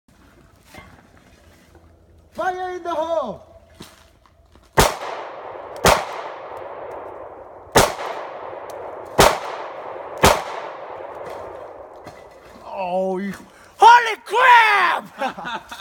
Beaver, PA
Kiwon's first time shooting a pistol, 45mm is too much!